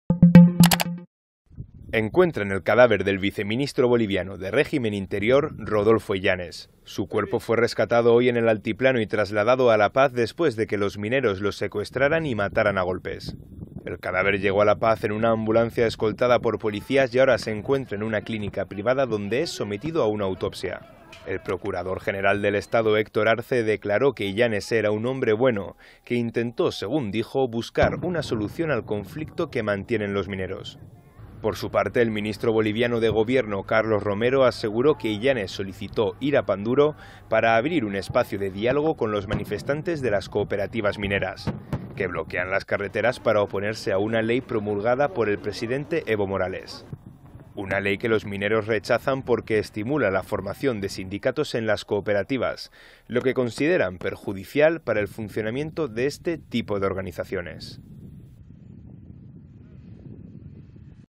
El Viceministro Rodolfo Illanes es asesinado en una protesta Minera
El Viceministro de Régimen Interior de Bolivia, Rodolfo Illanes, fue asesinado el jueves cuando estaba secuestrado por mineros que bloquean desde el último martes las carreteras del país andino, confirmó el ministerio del Interior.